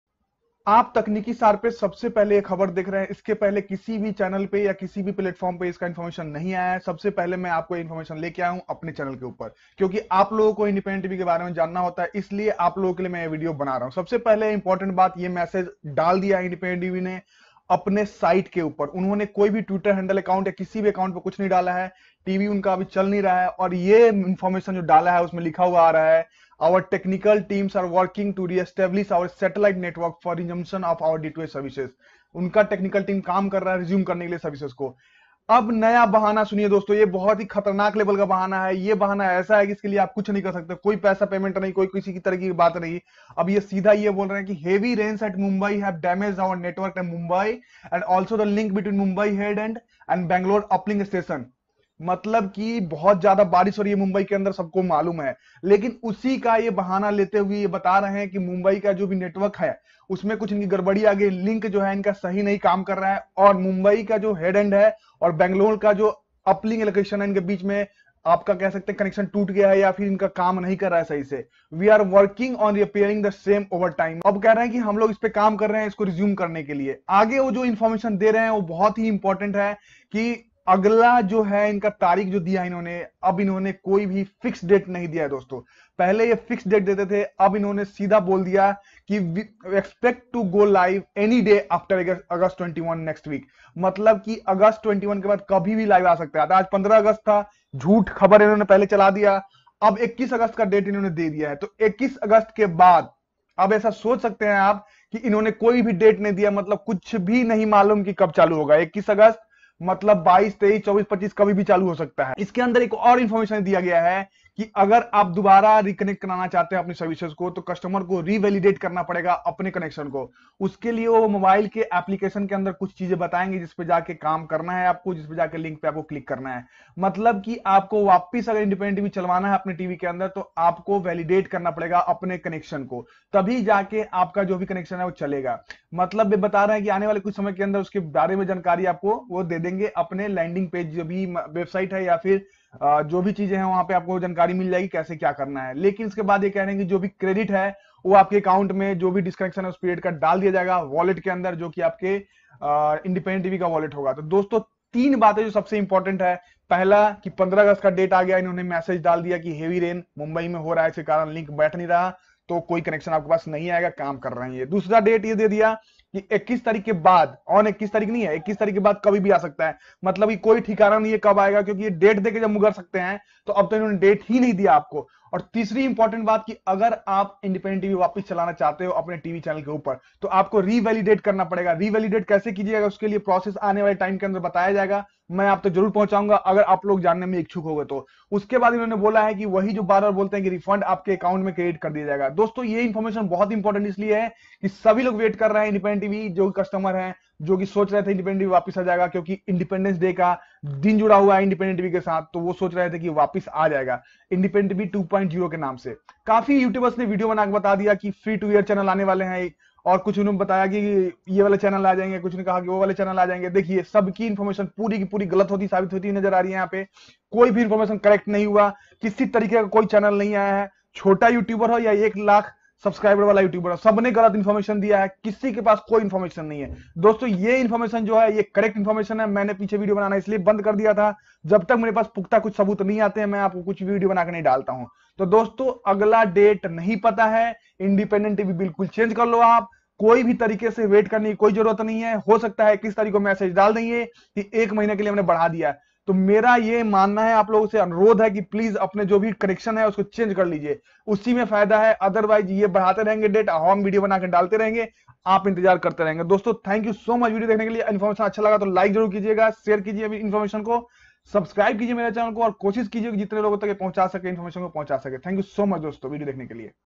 Wait till 21st August Now | Independent Tv Reconnection | Due to heavy Rain in Mumbai not resumed
Hello,

Dosto ek bar phir se independent tv ne phir se date change kr diya hai jo ki 15 august se badh ke 21 august ho gya hai. ismai kuch bahaut hi alag karan btaye hai inhone aur aap video ko last tak dekhiye independent tv ka sara information milega.

independent tv ke reconnection and due to heavy rain independent tv ke signals ke nahi aane ke bare mai ye video hai. Aapko is video mai jankari milegi ki aapke refund ke paise wapis aayenge ye nahi...

Jyda jankari ke liye aap mail kr sakte hai